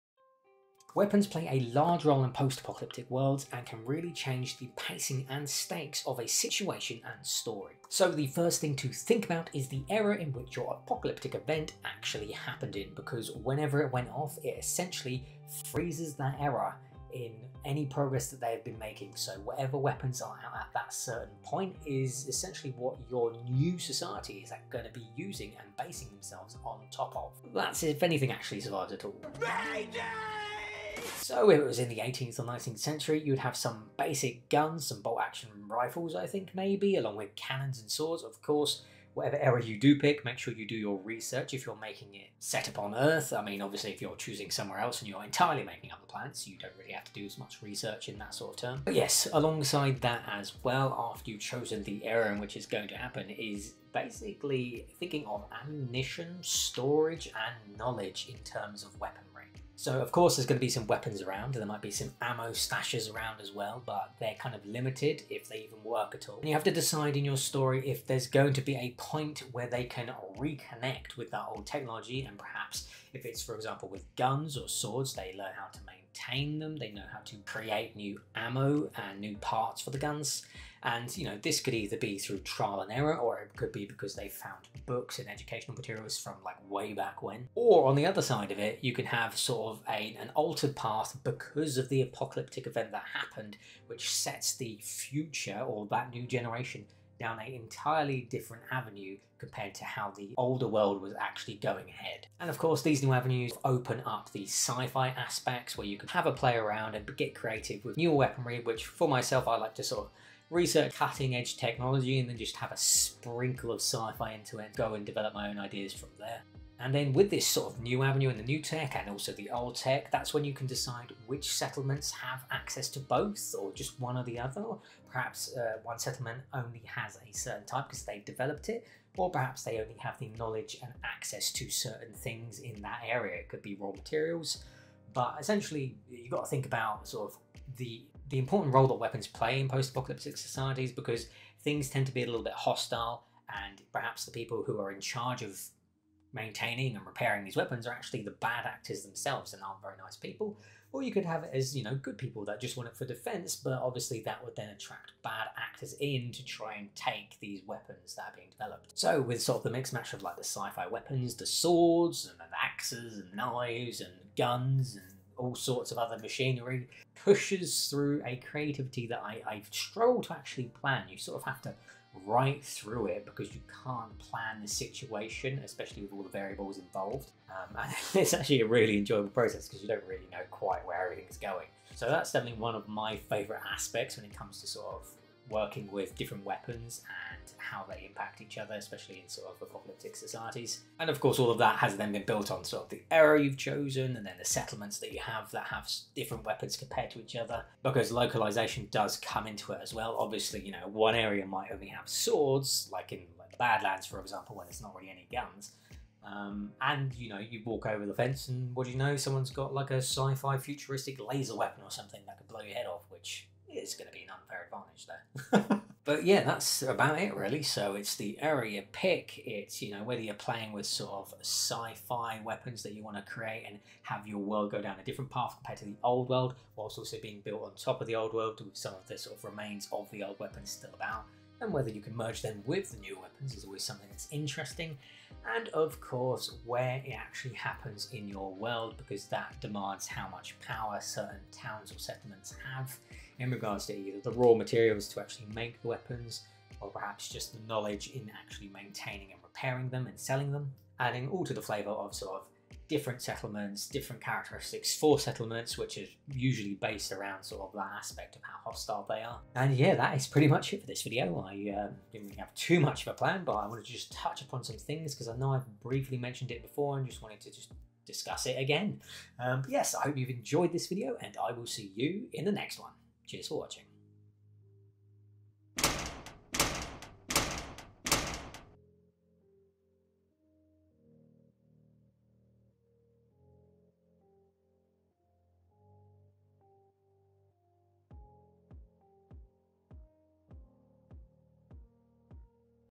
Weapons & The Apocalypse - Post Apocalyptic Writing Advice
In this brief video I touch upon everything I tend to consider when creating a post apocalyptic world in terms of weapons and what will be in it. From the apocalyptic event which causes the issues and freezes progress for weapon development, alongside finding stashes of old guns. Alongside this, knowledge can be forgotten, or progress can be furthered or altered in direction (something that can easily allow us to add that little bit of sci-fi flavour if it hasn't already).

Anyway, though I say it's more in-depth, it really is still just a bit of a ramble from myself over how I tend to think about and consider these things. Hopefully it might spark something for you :)

Sign up to my newsletter and receive a FREE PDF of A Novel Idea

AMAZON UK
AMAZON US